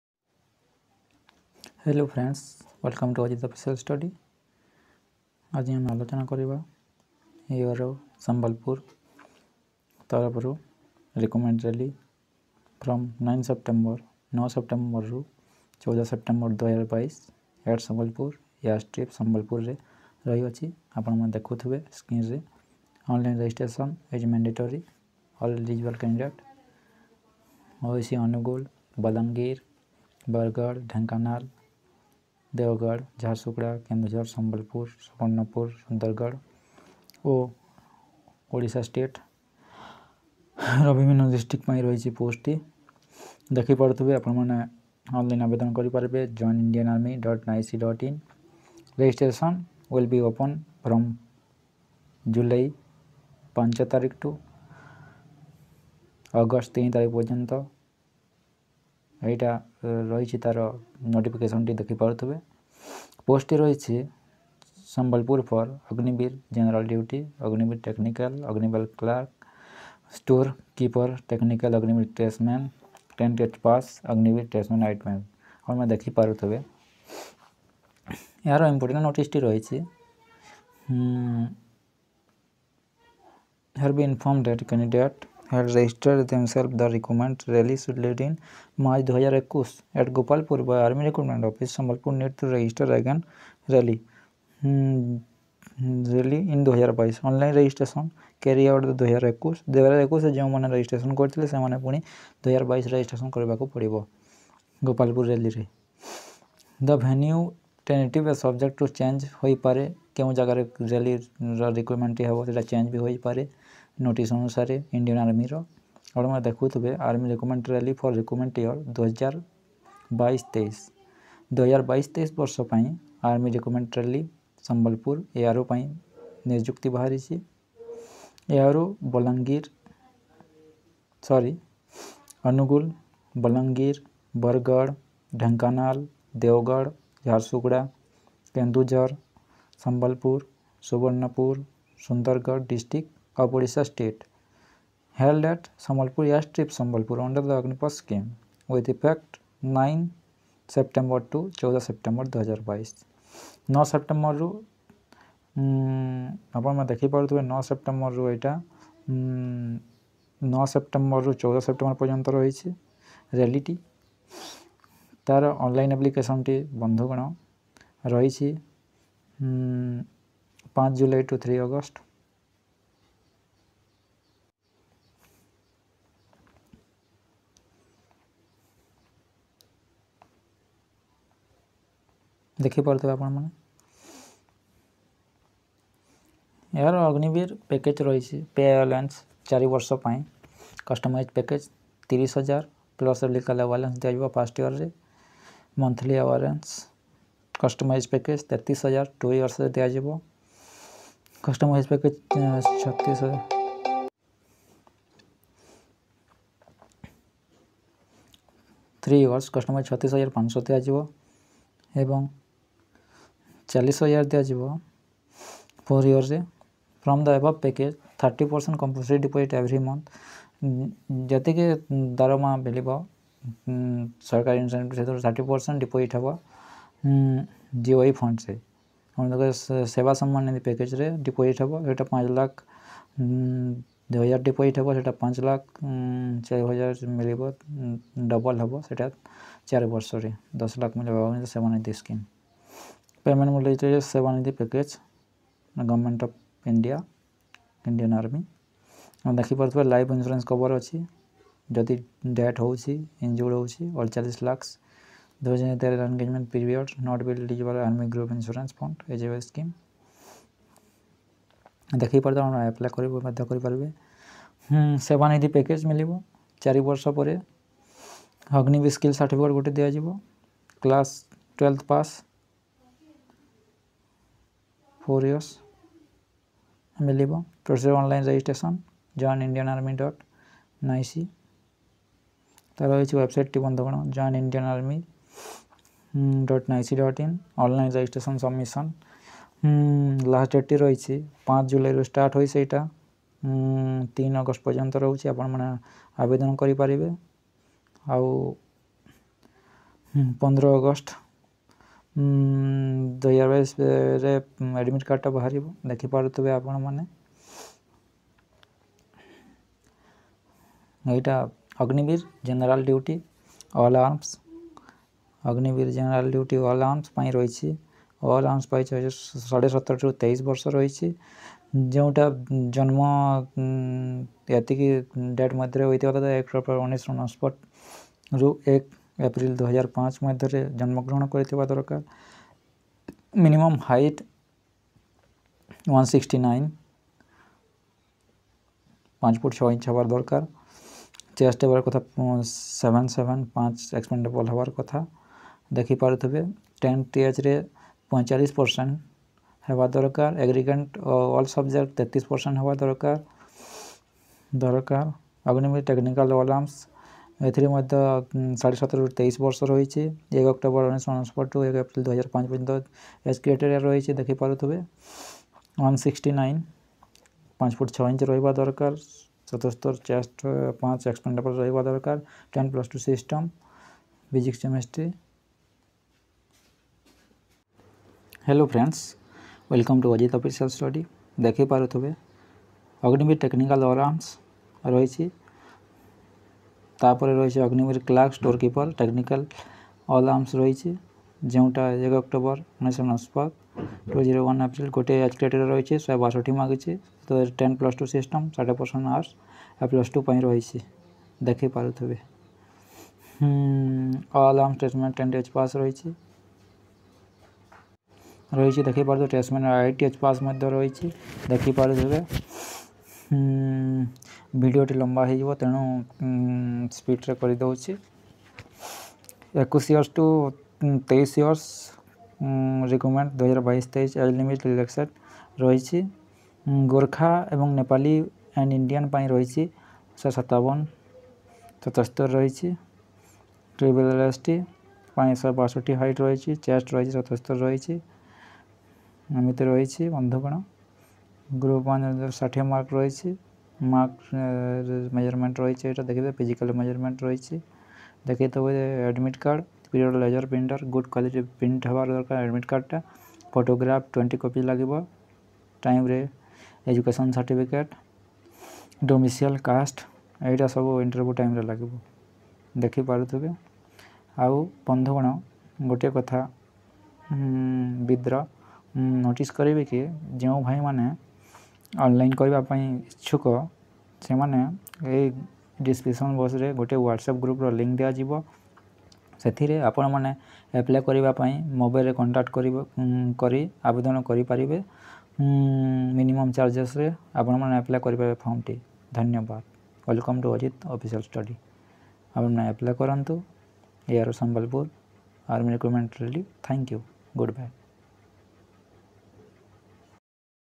ARO Sambalpur Army Rally 2022 Notification army Rally odisha 2022 sambalpur cuttack ONLINE APPLY
India Army Recruitment Rally is an annual rally organised by The Indian Army Recruitment Board in all states for the selection of candidates for various posts Zone-wise. Those candidates who want to make their career in Indian Army can appear in these open recruitment rally according to rally schedule.
whatapps link for apply low cost - join now
AJIT OFFICIAL STUDY-